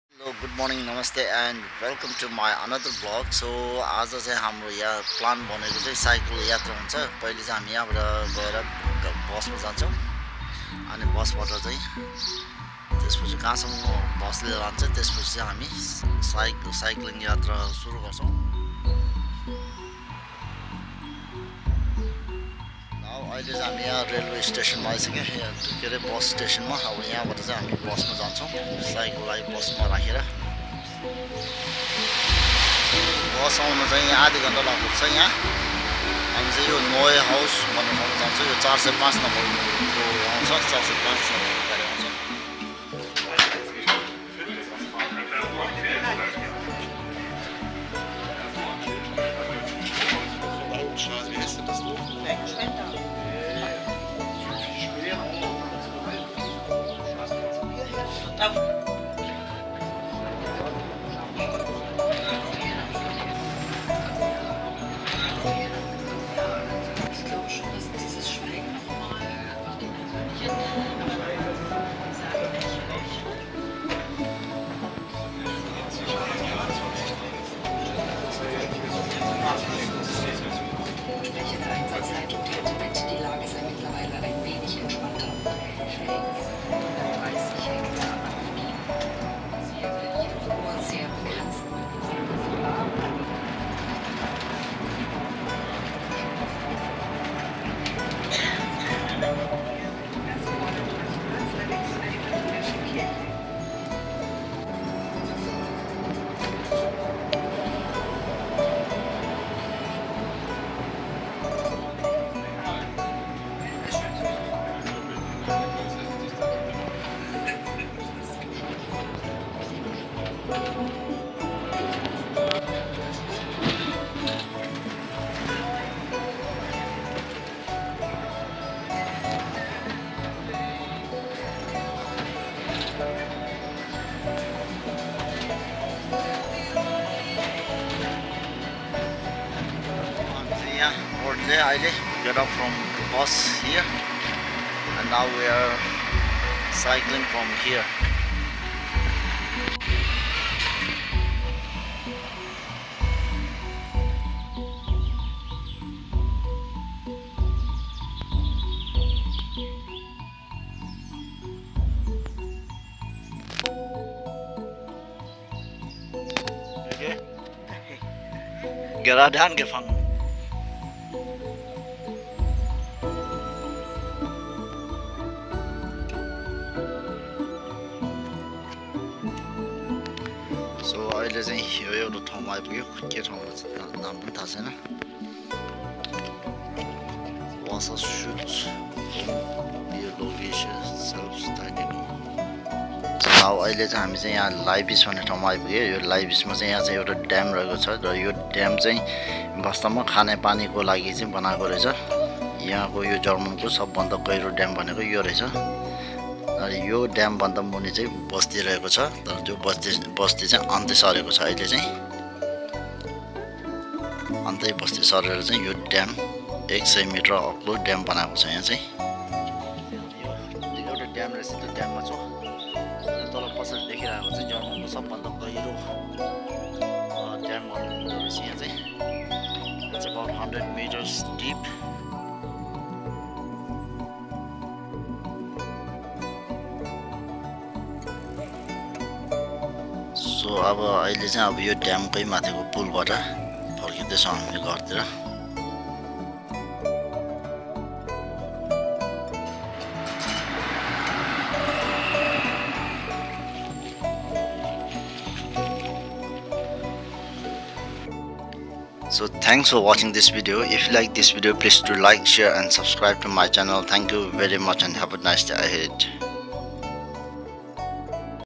Pedaling through Time | Tracing the History and Culture of Thüringen | GS Travel Vlog
Welcome to GS Travel Vlog.

Unveiling a hidden gem nestled in the heart of Thüringen, Germany, Leibis Reservoir stands as a testament to the captivating beauty of nature. For adventure enthusiasts and nature lovers alike, a cycling tour to Leibis Reservoir offers an unforgettable journey through picturesque landscapes, serene waters, and a wealth of cultural and historical wonders.